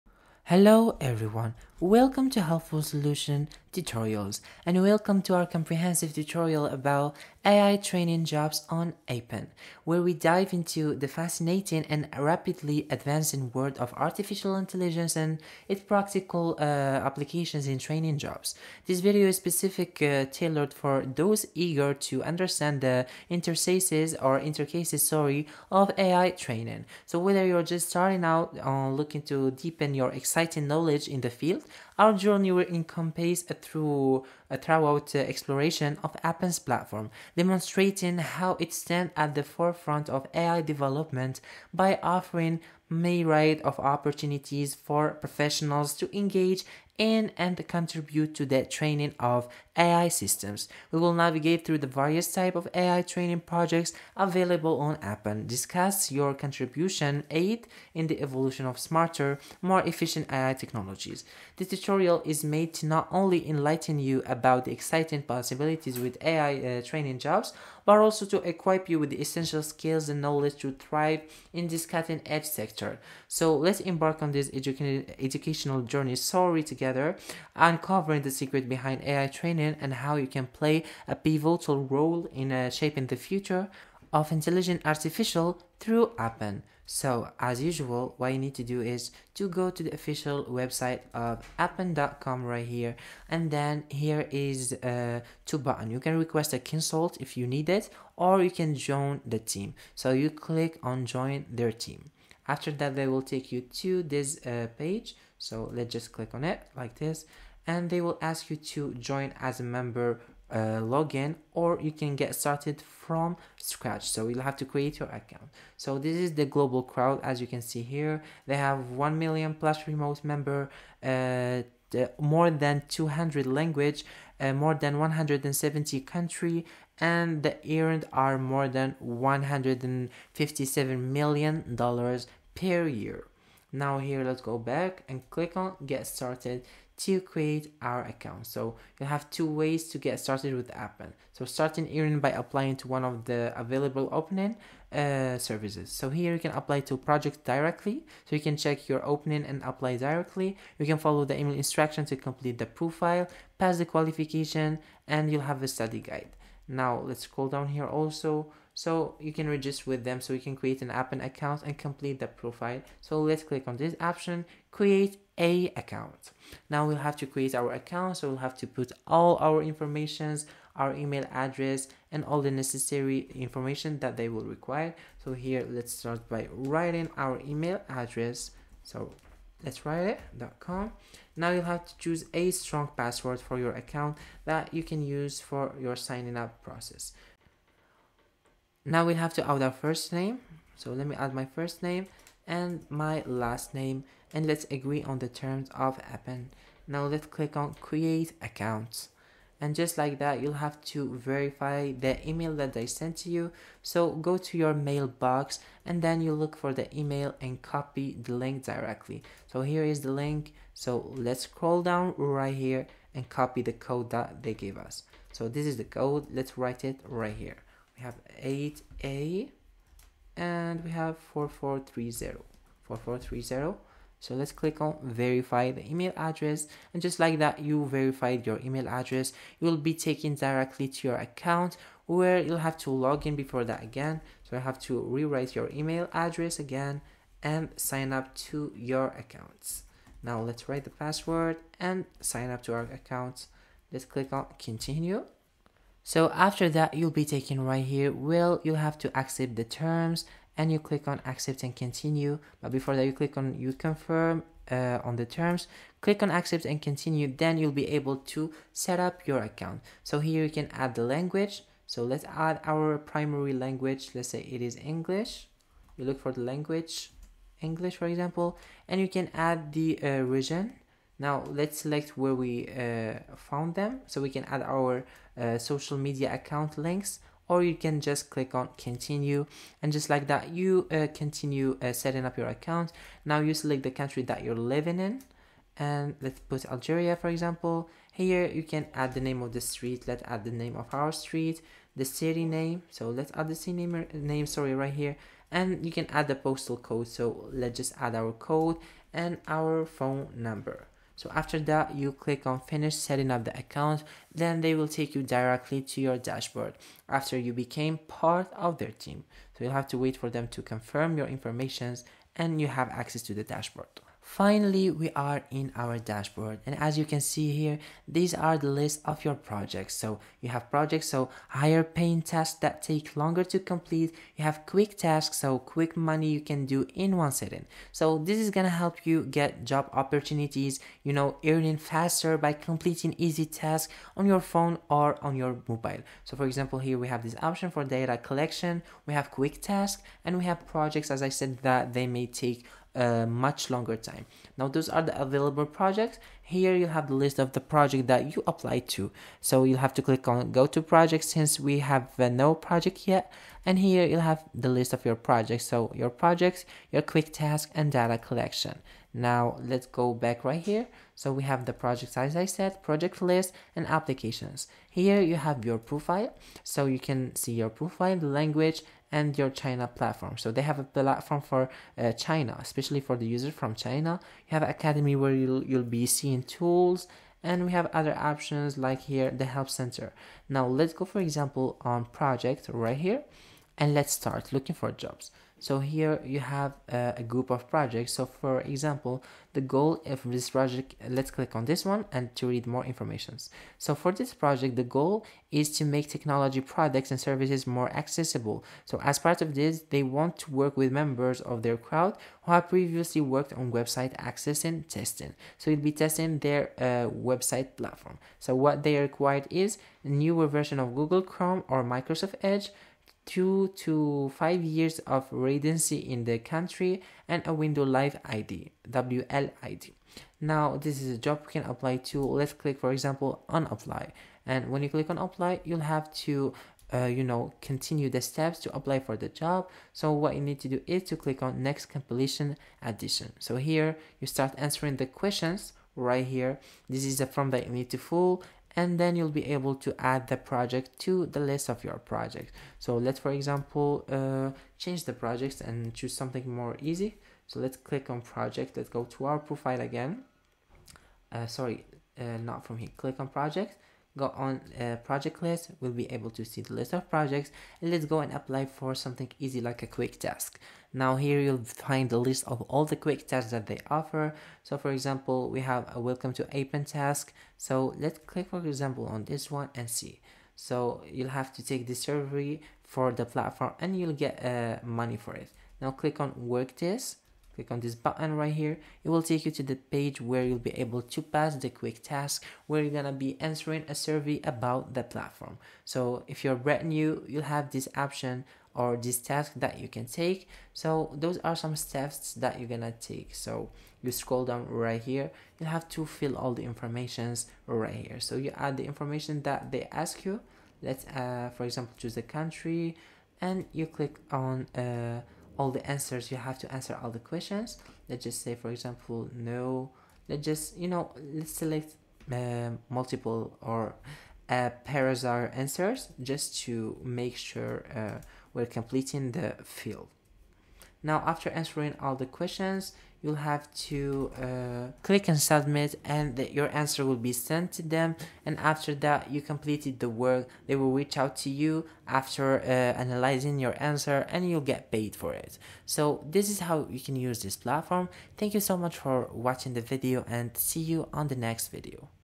Appen Tutorial 2024|How To Set Up & Use Appen (Step By Step)
Appen Tutorial 2024|How To Set Up & Use Appen (Step By Step). In this video tutorial I will show how to set up & use Appen.